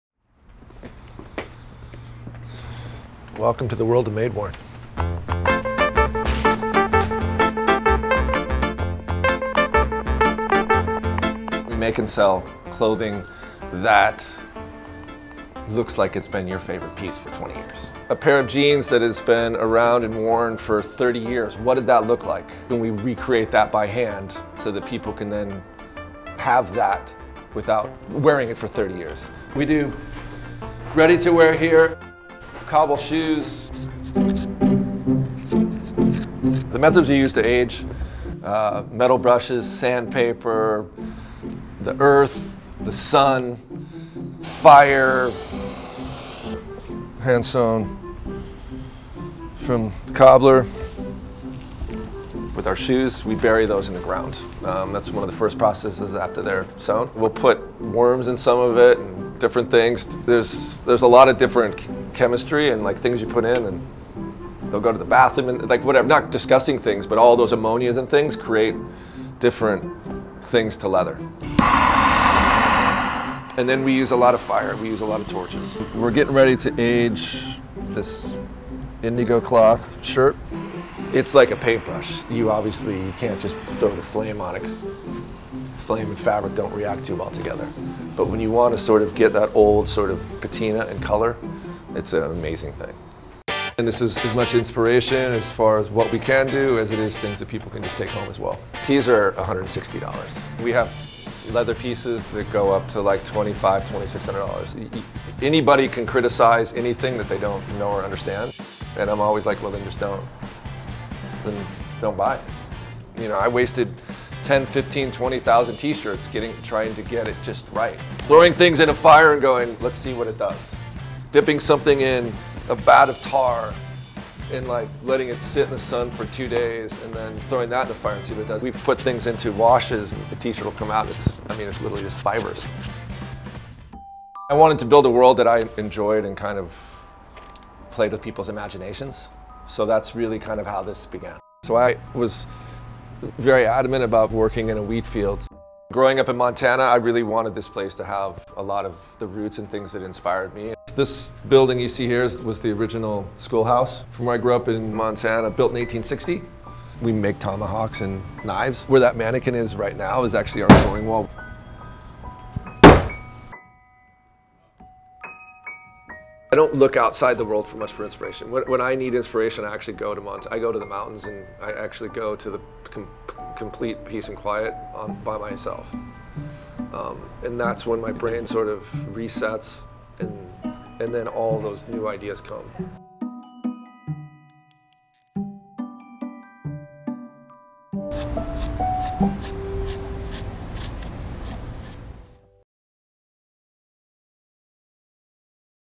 Making New Clothing Look Old With Blowtorches and Sandpaper
Los Angeles-based MadeWorn uses blowtorches, sandpaper and other techniques to produce clothing with that really worn-out look. Company founder Blaine Halvorson shows how they do it. Photo/Video: Mike Kofsky for The Wall Street Journal